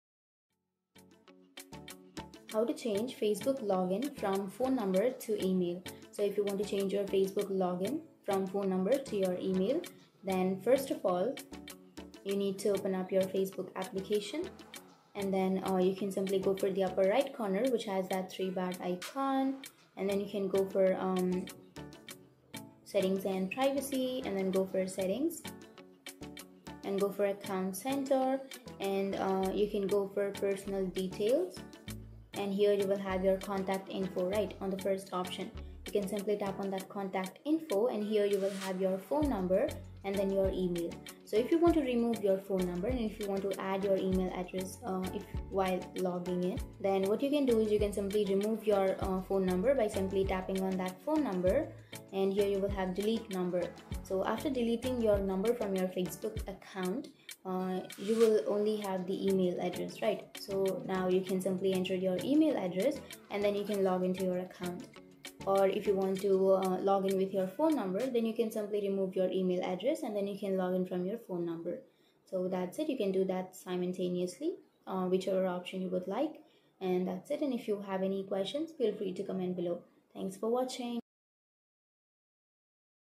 How To Change Facebook Login Phone Number To Email?
Welcome to App Mastery! In this step-by-step tutorial, we'll guide you through the process of updating your Facebook account settings. Learn how to make this change seamlessly and keep your account secure. Join us and take control of your Facebook login preferences.

Timestamps:
00:12 - Introduction to Changing Facebook Login Phone Number To Email
00:35 - Accessing Facebook Account Settings
01:10 - Switching Phone Number to Email for Facebook Login